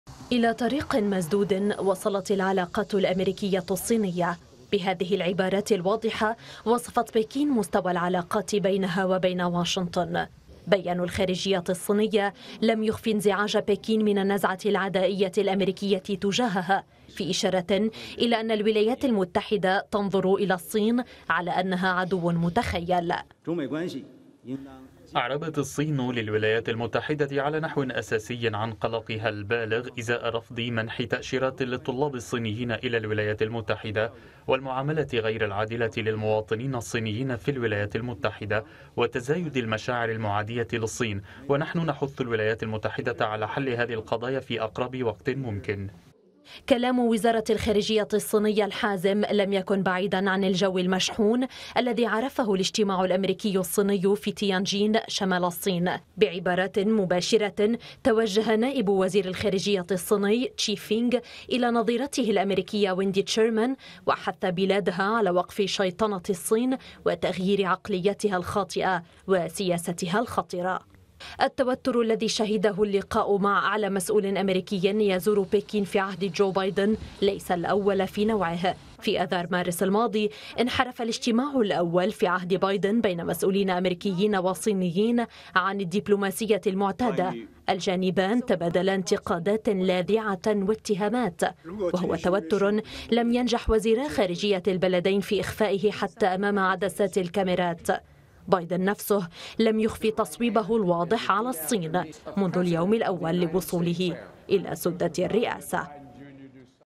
الخارجية الصينية: العلاقات مع واشنطن وصلت إلى طريق مسدود
إعداد: مريم عثمان

تقرير